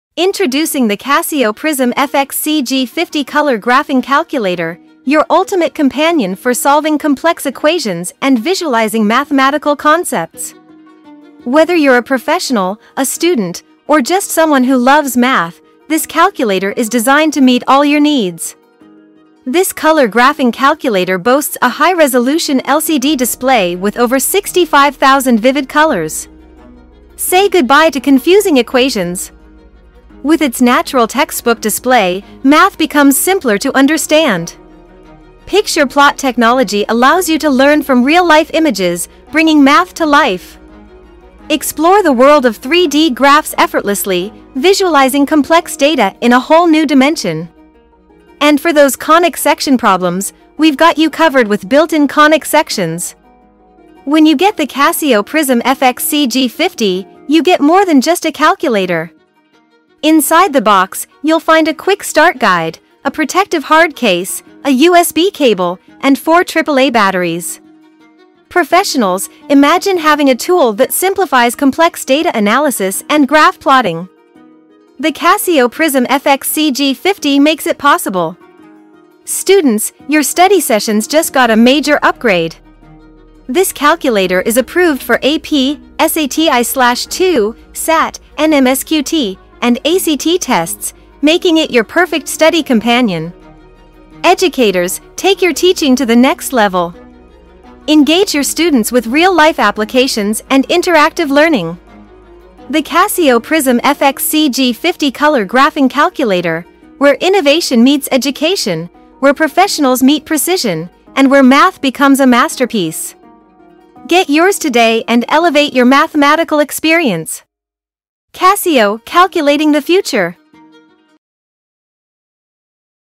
Casio PRIZM FX-CG50 Color Graphing Calculator Review - Transform Your Math Game!
Are you ready to take your math game to the next level? Look no further than the CASIO PRIZM FX-CG50 Color Graphing Calculator. In this comprehensive review, we dive deep into what makes this calculator a must-have for professionals, students, and anyone who loves math.

🌈 Vivid Color Display: With over 65,000 colors, this calculator's high-resolution LCD display brings your equations and graphs to life. Say goodbye to bland, monochrome calculators!

📚 Natural Textbook Display: Complex math problems are a breeze with the natural textbook display. It's like having a math teacher in your pocket.

🌆 Picture Plot Technology: Learn from real-life pictures and visualize mathematical concepts in a whole new way. Math becomes not just understandable but also enjoyable.

🔄 3D Graphing: Explore 3D graphs effortlessly, gaining insights into complex data like never before.

📈 Built-in Conic Sections: Conic section problems? No worries. This calculator has them built-in, making your math sessions stress-free.

Inside the box, you'll find a Quick Start Guide, a Protective Hard Case, a USB Cable, and four AAA batteries, ensuring you're ready to tackle math challenges right away.

But it's not just for students. Professionals can benefit from its data analysis capabilities and precision graph plotting. It's approved for AP, SAT I/II, PSAT, NMSQT, and ACT Tests, making it a valuable tool in any math-related profession.

Teachers, take note! This calculator can revolutionize your teaching. Engage your students with interactive learning and real-life applications.

Join us in this review to explore how the CASIO PRIZM FX-CG50 Color Graphing Calculator is transforming the world of math.